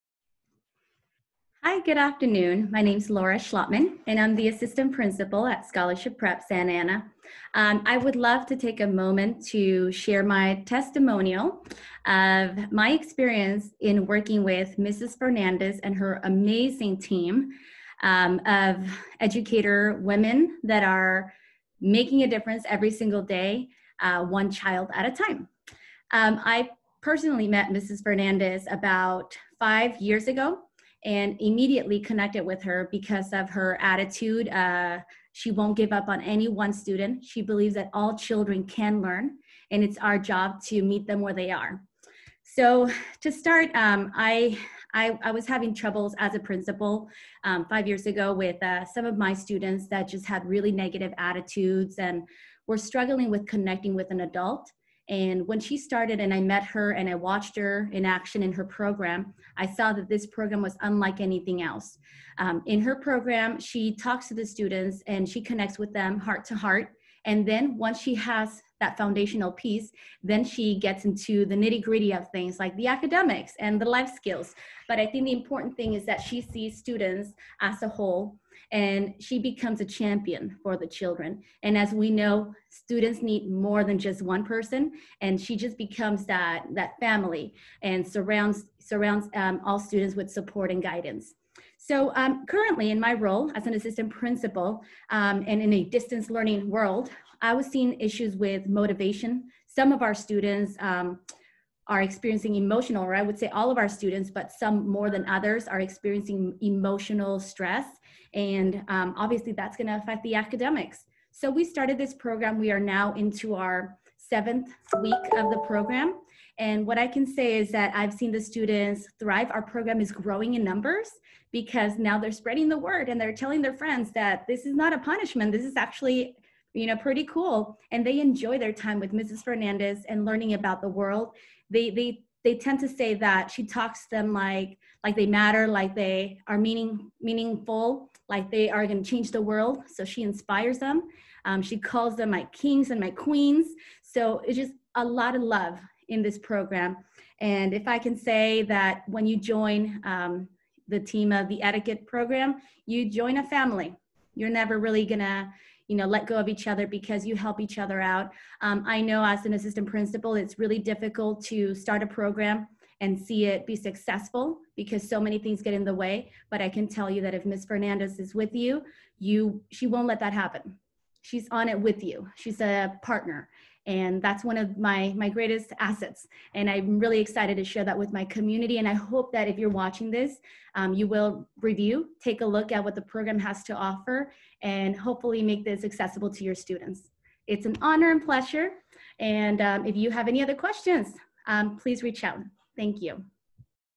Laura Schlottman, Assistant Principal | Imagine Etiquette Testimonial
Mrs. Laura Schlottman, Assistant Principal of Scholarship Prep Charter School, Discusses Her Experiences With Imagine Etiquette's SEVA™ and VASE Enrichment Programs For Schools.